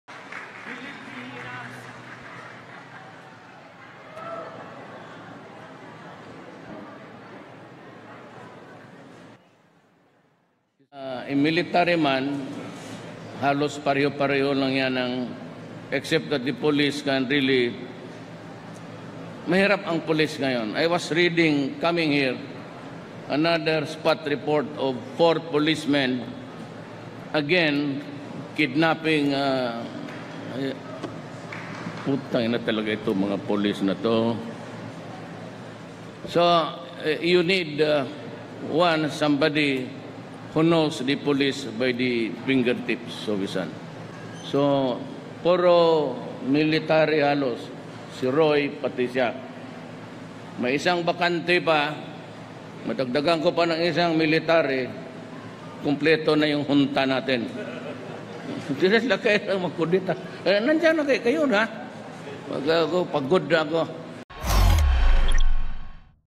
Duterte se endurece y designa militares al frente de dos ministerios
El presidente de Filipinas, Rodrigo Duterte, cuestionado internacionalmente por las consecuencias de su guerra contra las drogas, designó al general Eduardo Año, actual jefe de las Fuerzas Armadas, como ministro del Interior, días después de nombrar a otro militar al frente de Medio Ambiente.

Año, de 55 años, tomará en octubre las riendas del Departamento del Interior y Gobiernos Locales, tras dejar la jefatura de las Fuerzas Armadas, anunció Duterte durante una conferencia de prensa en Manila previo al inicio de una gira por varios países de la región.